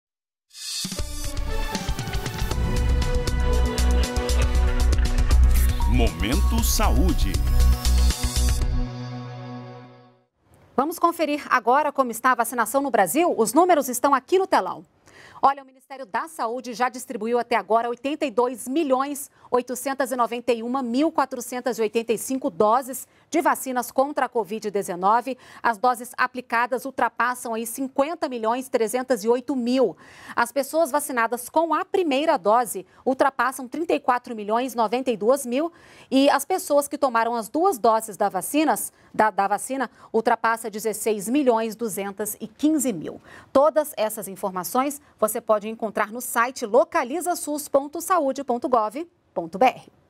Vacinômetro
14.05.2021 - O Ministério da Saúde já distribuiu 82 milhões de doses de vacinas contra a covid-19.